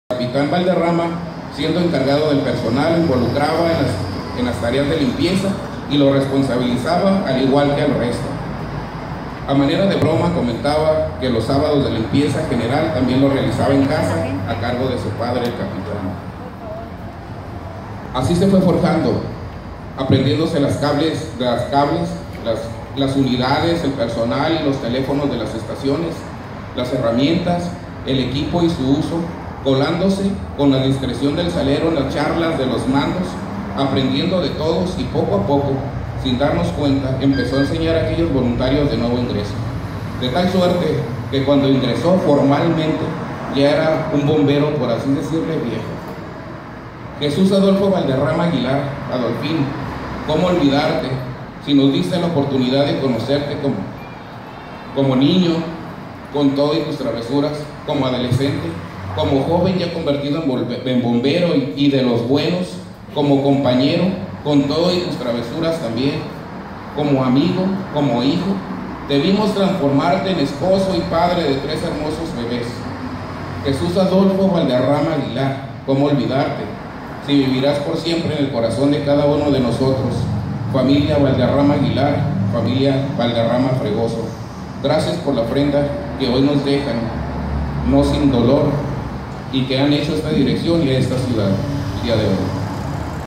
Con profunda admiración el bombero Martin Reyes define al su compañero Adolfo Valderrama
Conteniendo el dolor que implica la pérdida, Martin Reyes nos muestra en una breve intervención quién era Adolfito, el bombero muerto en cumplimiento del deber.